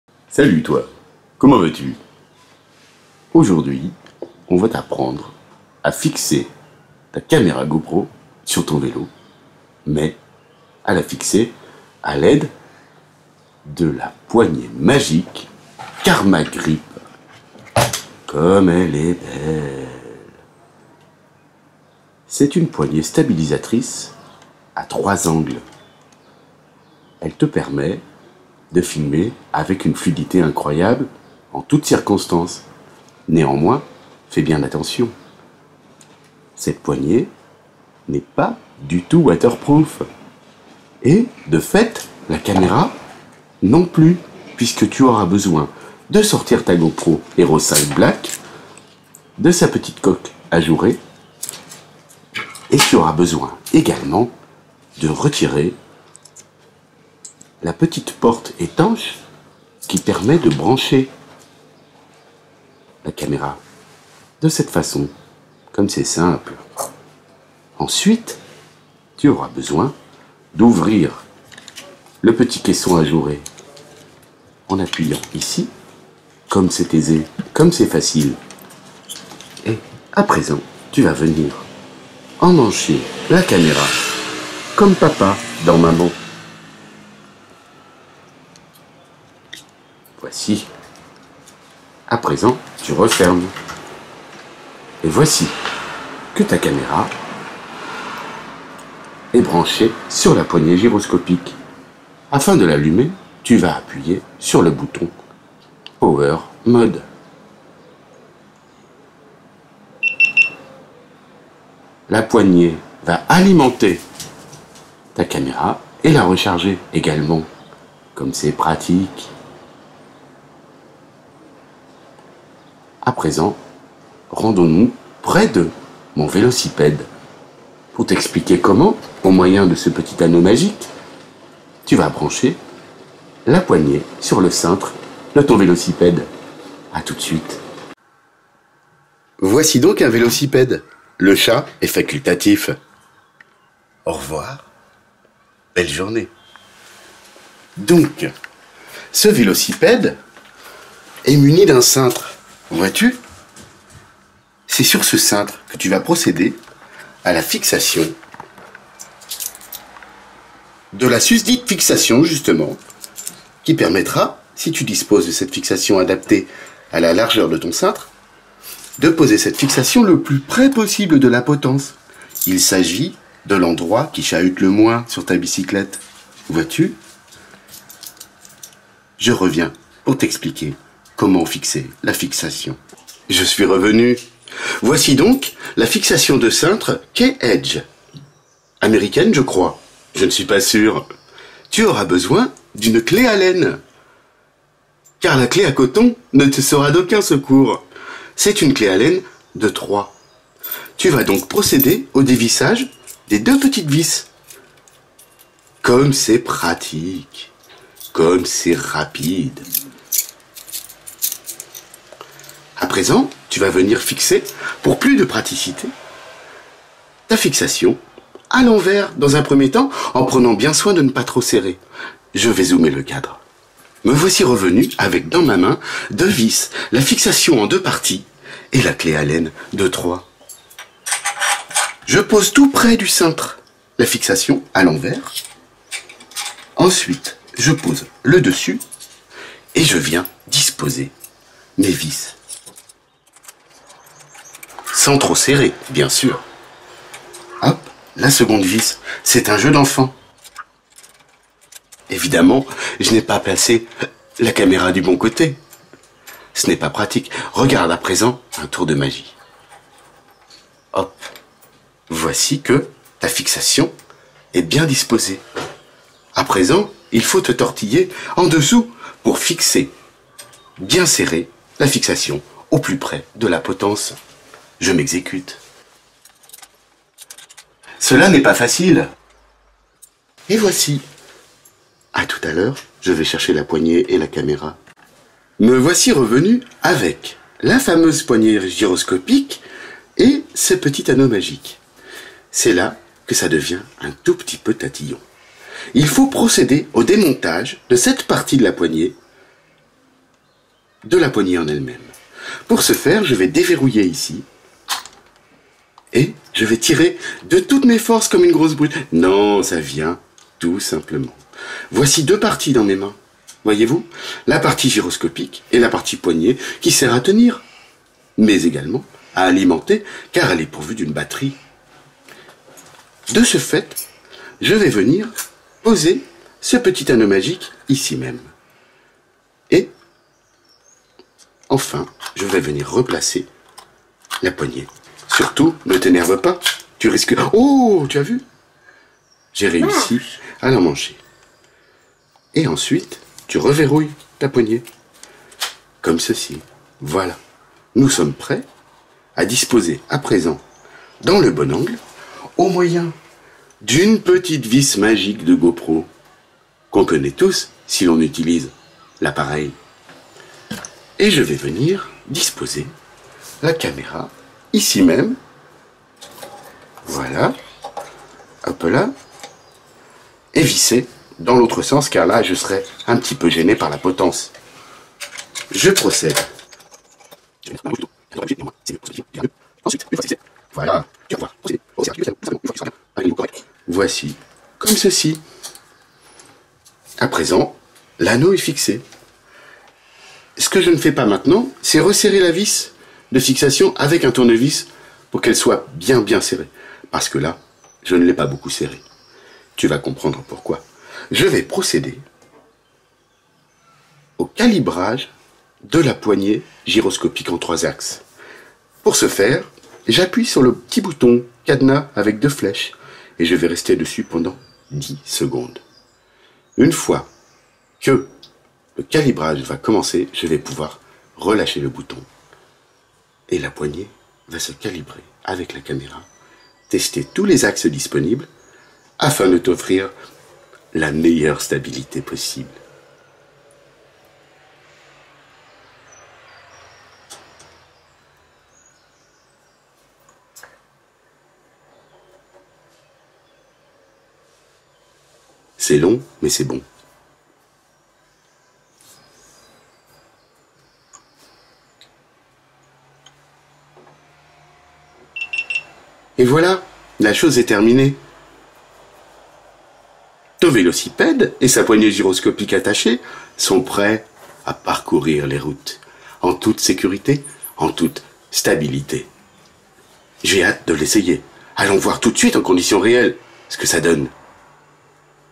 [TUTO] Installer une GoPro avec poignée Karma Grip sur un cintre de vélo
Salut, ami vidéaste amateur de bicyclette, suite à pas mal de questions posées sur cette même chaine, j'entame une série de tutoriels "conseils" qui visent à expliquer la manière dont je procède pour capturer ces instants magiques de déplacements à vélo :)

Voici comment j'ai procédé pour l'installation d'une GoPro 5 dans la poignée gyroscopique "Karma Grip" de GoPro, sur le cintre de mon vélo de route. Il faut disposer d'une fixation "guidon" de vélo, d'un diamètre adapté à celui du tube.